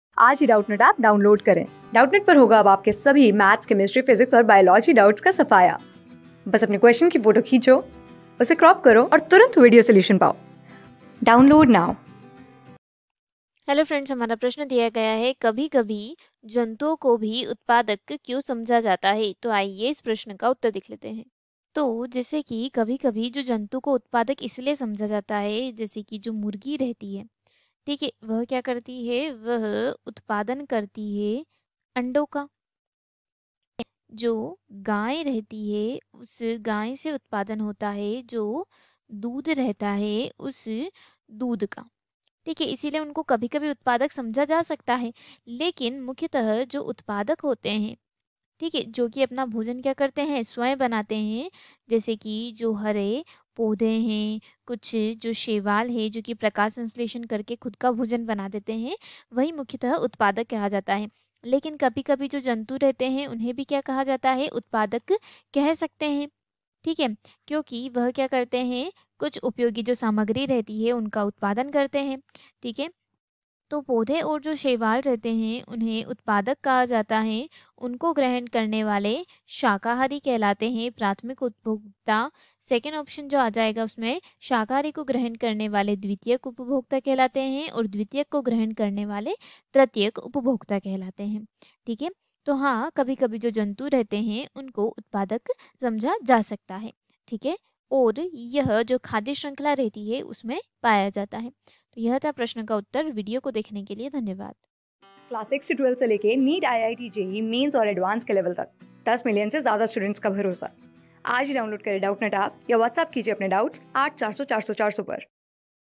कभी-कभी जंतुओं को भी उत्पादक क्यों समझा जाता है? | CLASS 9 | प्राकृतिक संसाधन | BIOLOGY | LAKHM...
कभी-कभी जंतुओं को भी उत्पादक क्यों समझा जाता है?
Class: 9
Subject: BIOLOGY
Chapter: प्राकृतिक संसाधन
Board:FOUNDATION

👉 Have Any Query? Ask Us.